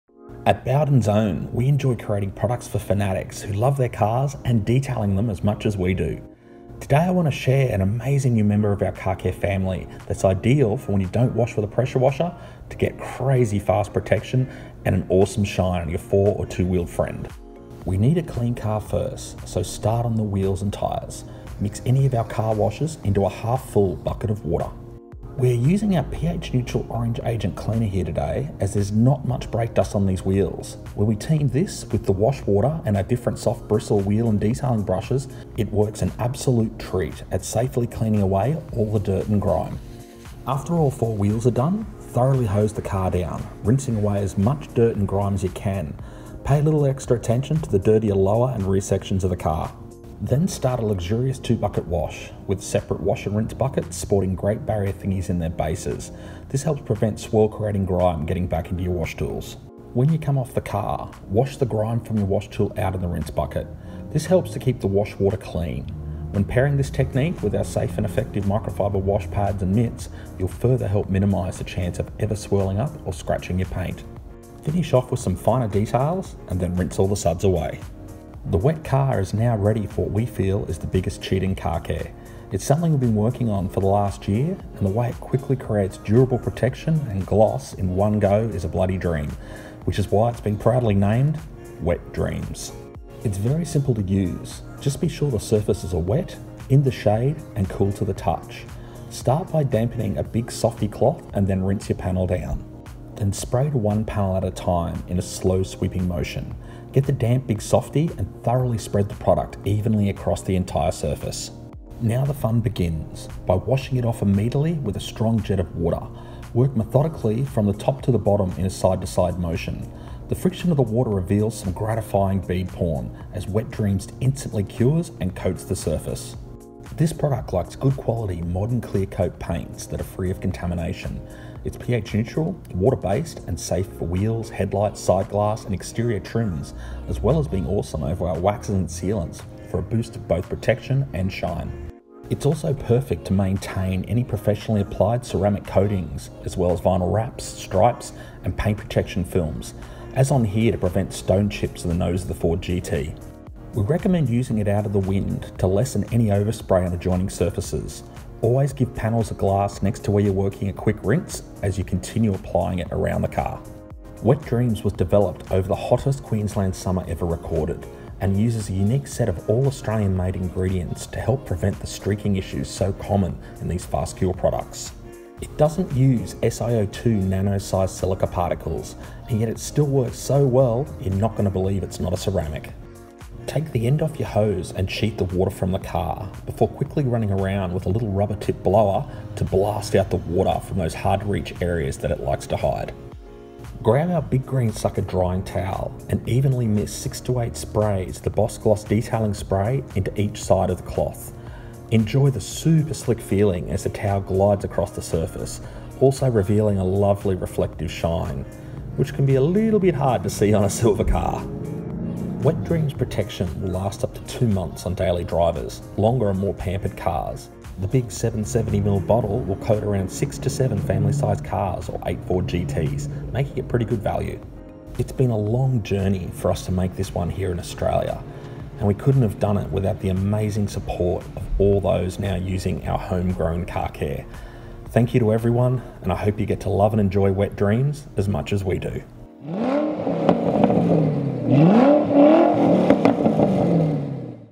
Wet Dreams - a great cheat in car care!
Our new super hydrophobic spray that creates almost instant protection on a wet car after washing. Just spray on, wipe and then rinse off for it to work like a dream! It's made for fanatics who don't always use a pressure washer when detailing, quickly creating a fantastic shine, durable protection with awesome bead porn and water sheeting effects. It does it so easily that you'll feel like you're cheating.
This all Aussie made and developed formula uses 100% locally made ingredients to get the "lotus leaf" hydrophobic effect that works that well, you won't believe it's not "ceramic".
Wet Dreams works best on modern clear coat paints, with the protection lasting up to two months on daily driven cars. You can use it over our existing waxes and sealants to give them a boost in shine and protection. Also fantastic for maintaining professionally applied ceramic coatings, as well as Paint Protection Film (PPF as seen on the nose of this Ford GT in the video) and vinyl wraps.

It's $34.99 AUD  for the bigger 770ml bottle and can be found at Autobarn stores currently, or special ordered in from other resellers from the start of July, with part code BODREAMS.

A big thank you to the great team at Autohouse Storage for allowing us to use the Ford GT at their fantastic facility in Brisbane.